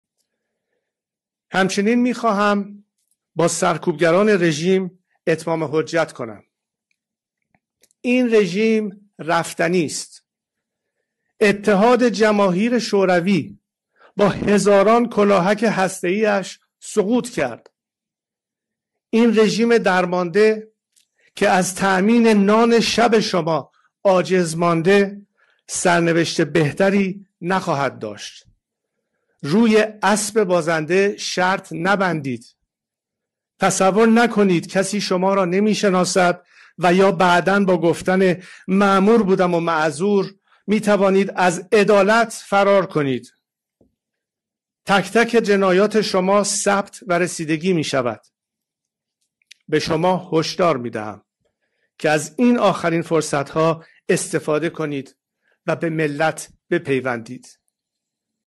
اتمام حجت شاهزاده رضا پهلوی به سرکوبگران رژیم/بازپخش بخشی از"سخنی باهم‌میهنان"/ فعال کردن گشت ارشاد
باز پخش بخشی از "سخنی با هم‌میهنان" شاهزاده رضا پهلوی بمناسبت فعال کردن گشت سرکوب ارشاد

همچنین می‌خواهم با سرکوبگرانِ رژیم اتمام حجت کنم:
"این رژیم، رفتنی‌ست. اتحاد جماهیر شوروی با هزاران کلاهک هسته‌ای‌اش سقوط کرد. این رژیم درمانده، که از تامینِ نانِ شب شما هم عاجز مانده، سرنوشت بهتری نخواهد داشت. روی اسبِ بازنده شرط نبندید! تصور نکنید کسی شما را نمی‌شناسد و یا بعدا با گفتن «مامور بودم و معذور»، می‌توانید از عدالت فرار کنید. تک‌تکِ جنایات شما ثبت و رسیدگی می‌شود. به شما هشدار می‌دهم که از این آخرین فرصتها استفاده کنید و به ملت بپیوندید."

سخنی_با_هم‌میهنان